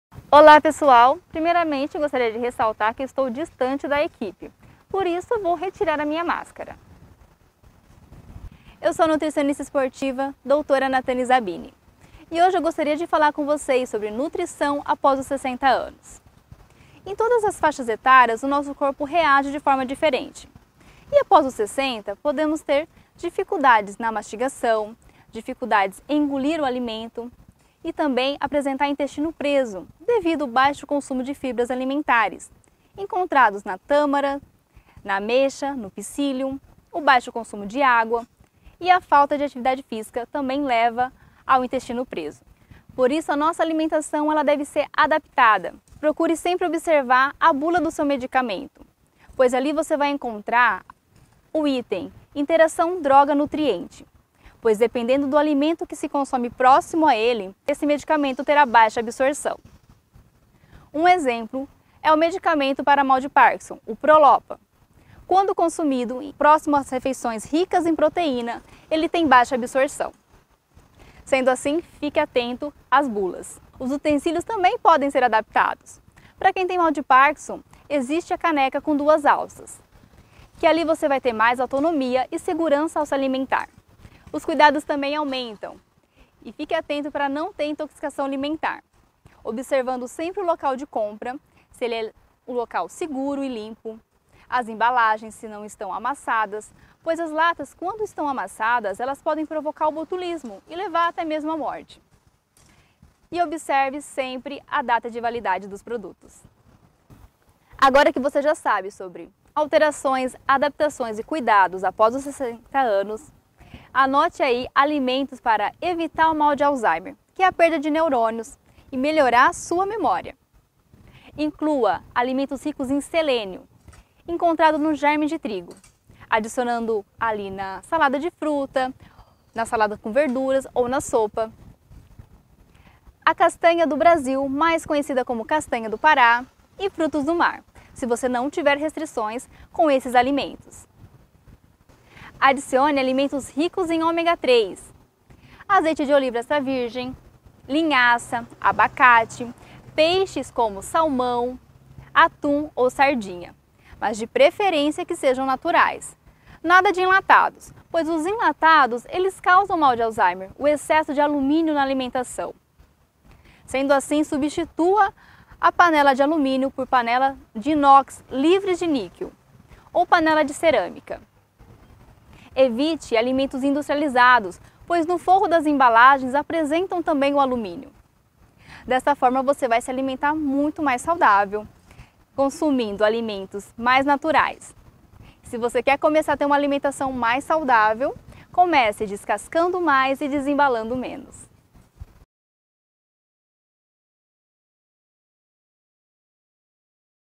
Nutrição após os 60 anos! PROGRAMA 60 MAIS - Clube 9
Dentro do Programa 60 Mais do Clube 9 de Julho, a Dra. Nathany Zabini apresenta dicas importantes para uma melhor alimentação e qualidade de vida. Confira!!

Imagens e edição: Carlos Cabiró